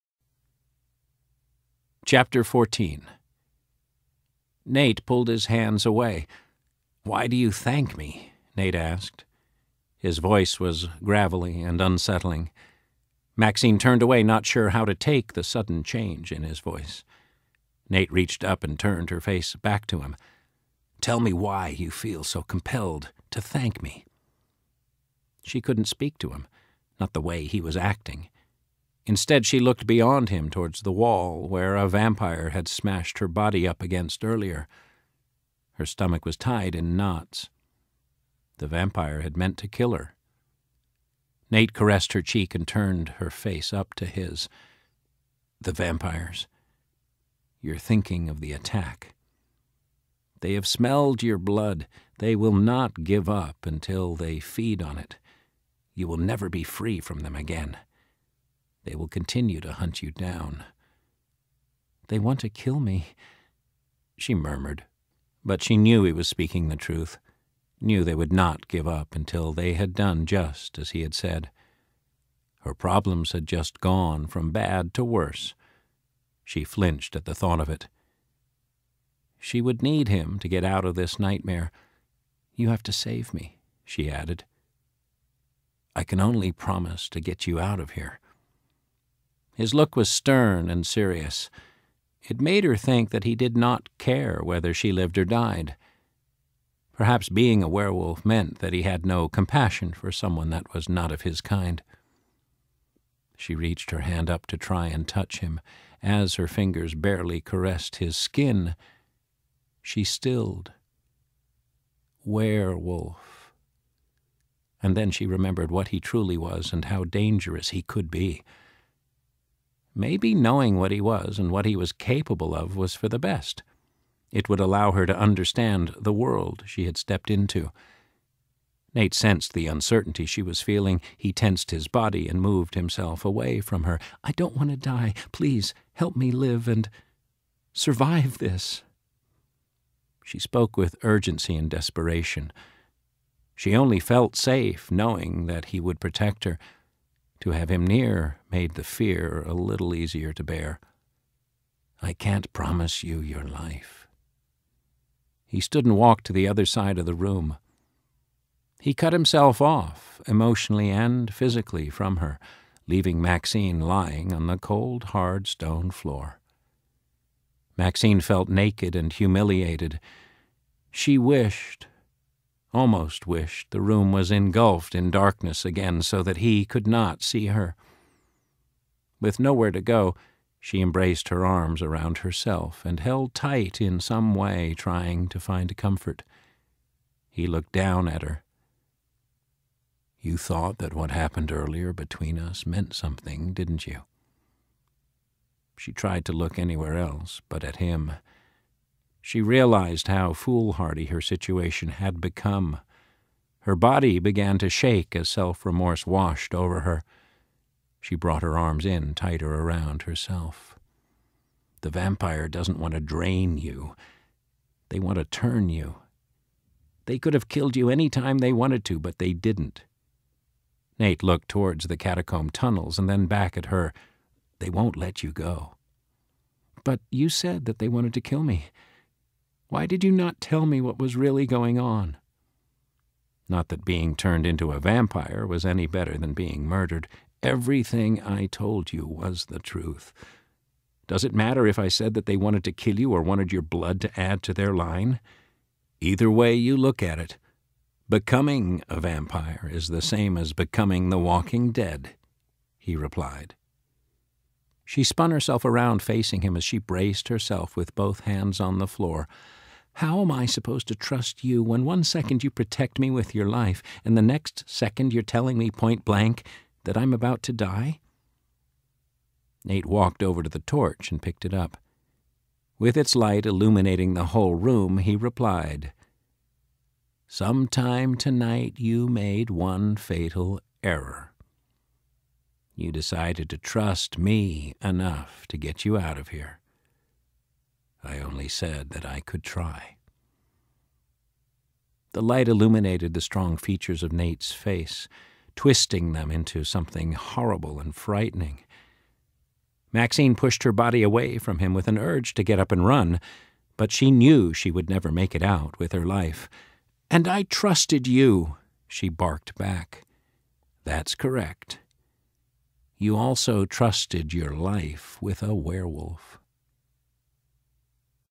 Blood Moon: Werewolves and Shifters (Audiobook)
Maxine Lewis finds herself thrown into a world she never knew existed. What she does not expect is that she will become the prey in a game of wits to stay alive.

Nate Jackson is the leader of a werewolf pack. Someone has been bringing the members to extinction. As their leader, it is up to him to find out who is behind the killings. This only proves to be more difficult to accomplish when Maxine is thrown into his protection. All the while, Maxine and Nate are being pushed further into the underground world of the vampires.

eBook and print versions

This is an excerpt from the audio book 'Blood Moon: Werewolves and Shifters.' The narrator is Mr. Alex Hyde-White. Audiobook can be found through iTunes, Audible and Amazon.